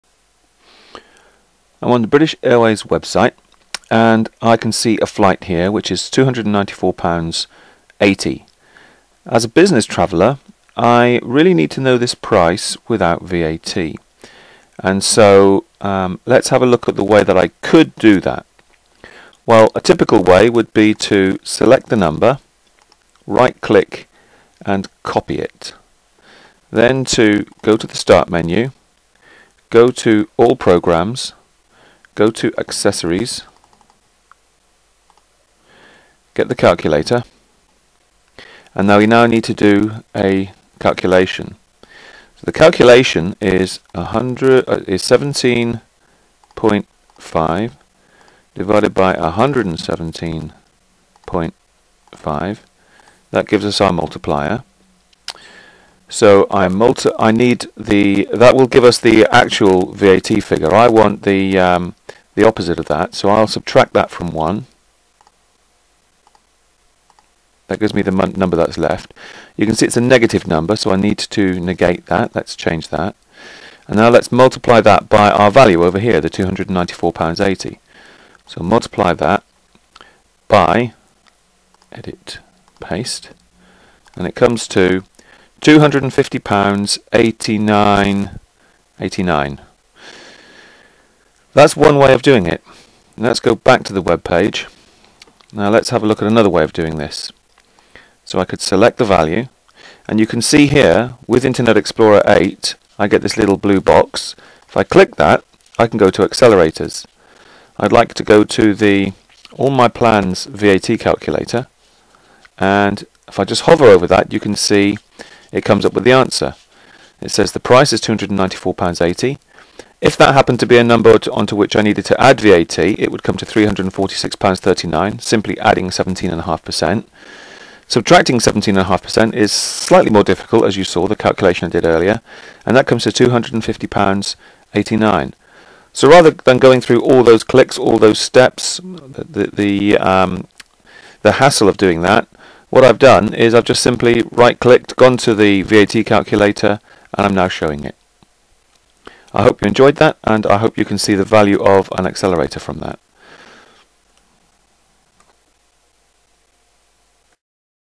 All My Plans VAT Calculator: IE8 accelerator
A video screencast of the All My Plans IE8 VAT Calculator.